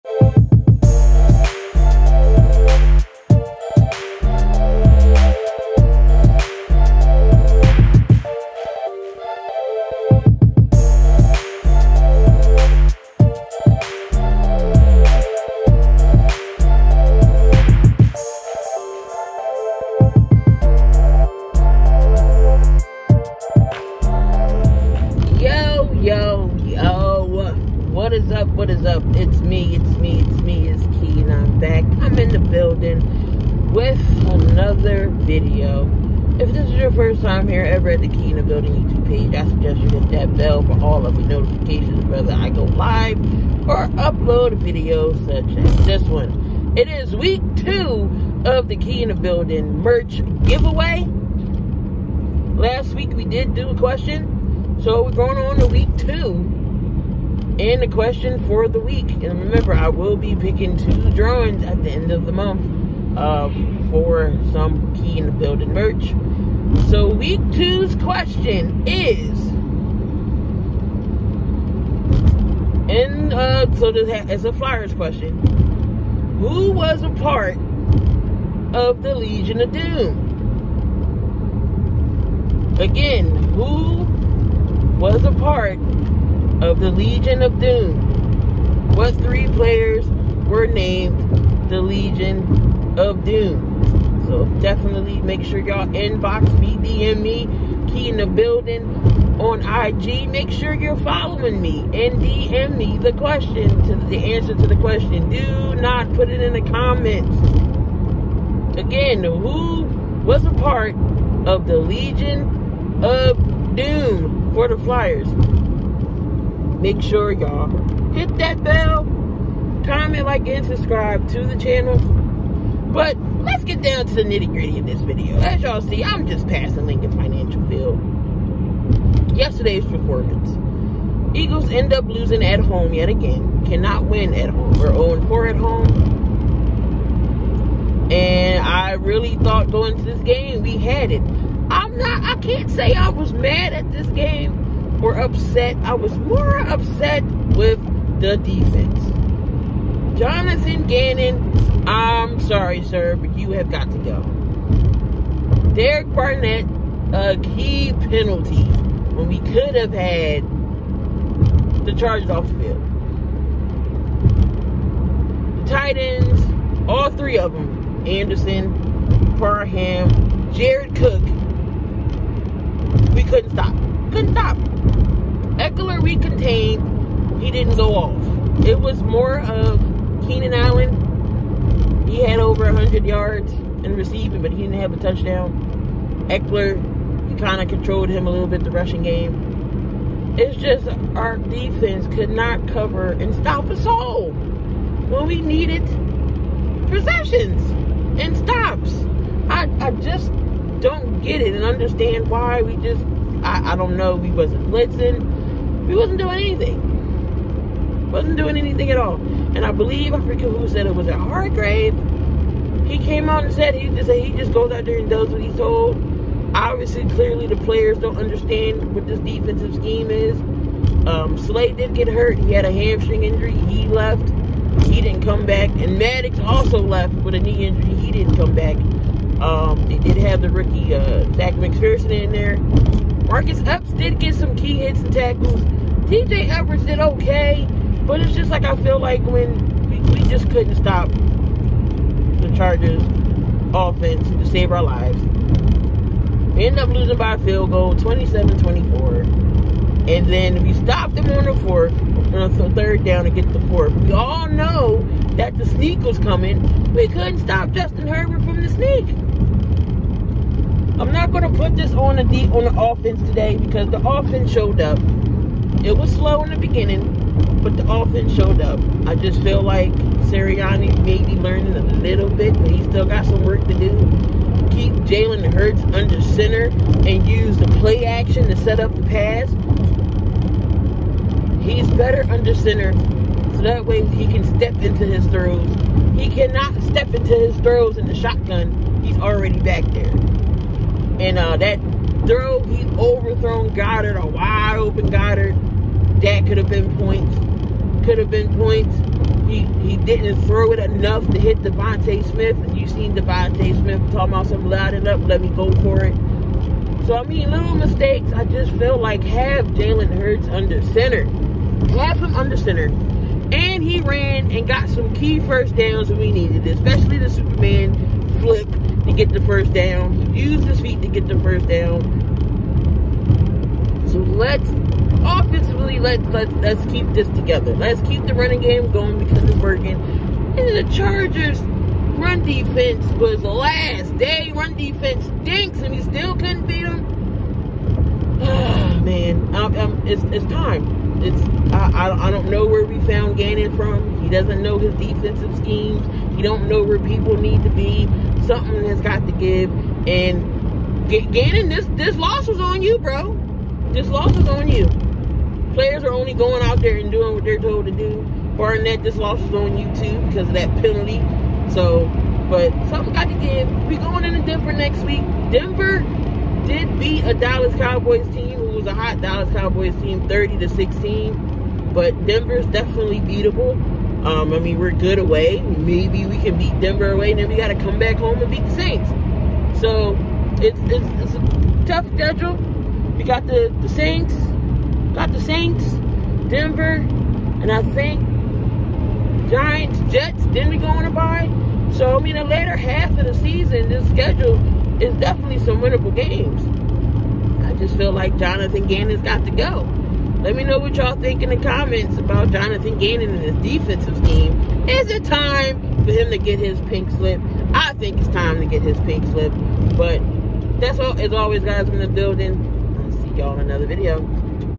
Gannon Needs The Pink Slip
The Philadelphia Eagles lose at home again 27-24 to the Chargers. Defense played horrible and Johnathan Gannon has to go